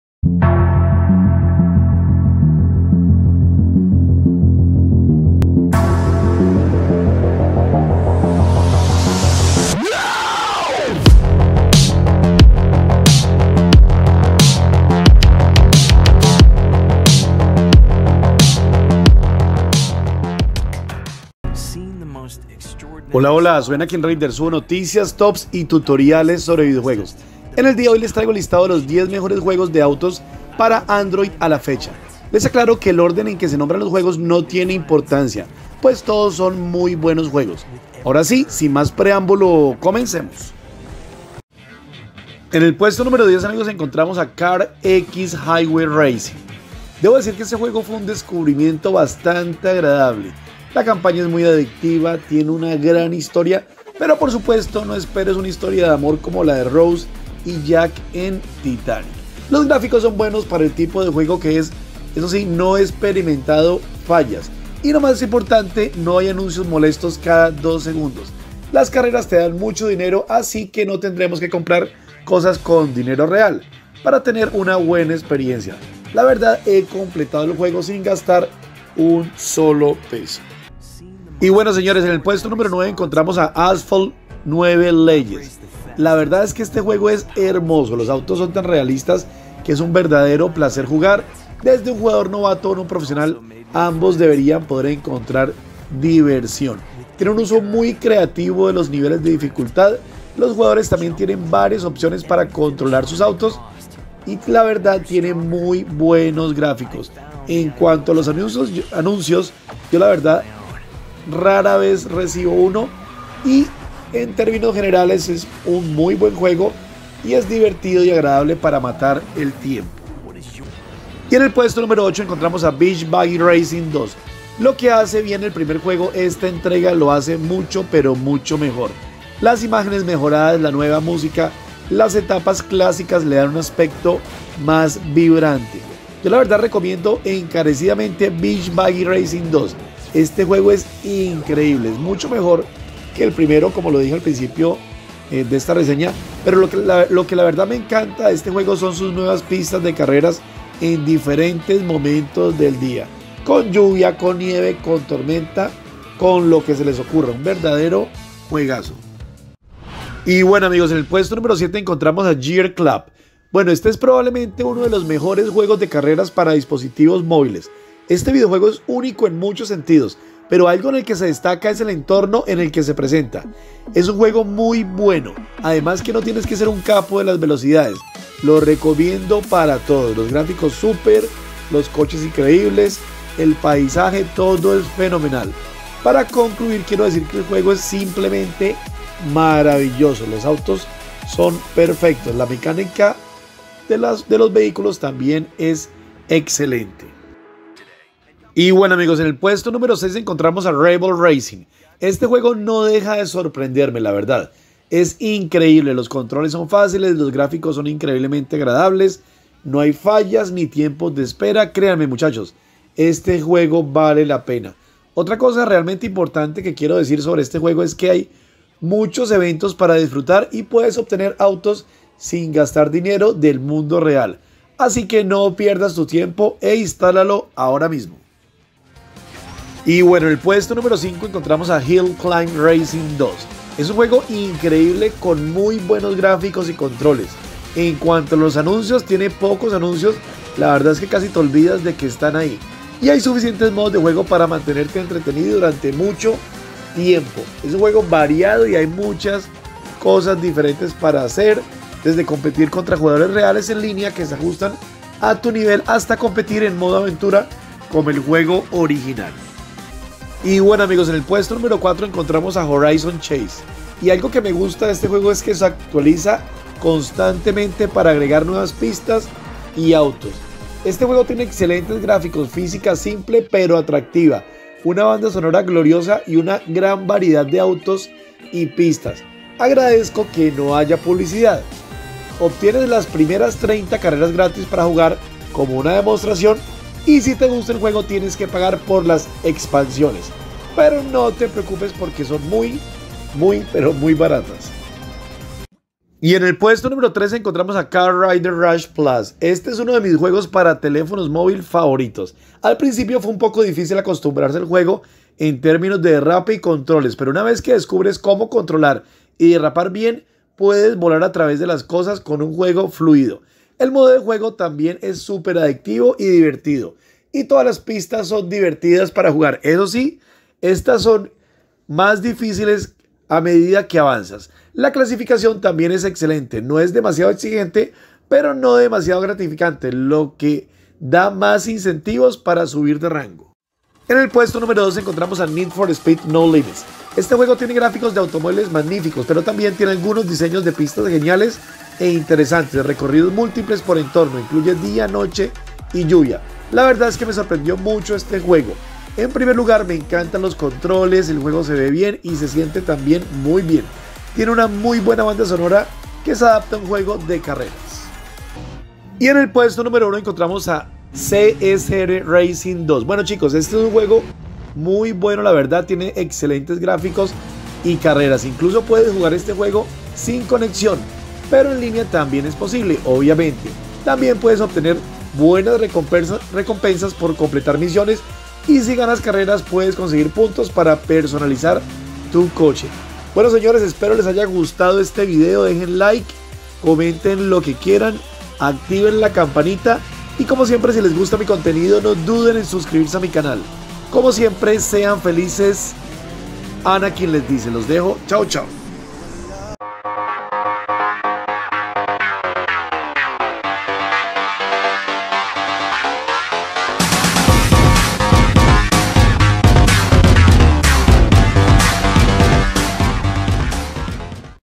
Juegos de CARROS para Android con Buenos Graficos ONLINE y OFFLINE 2022
Listado de los mejores juegos para celular en 2022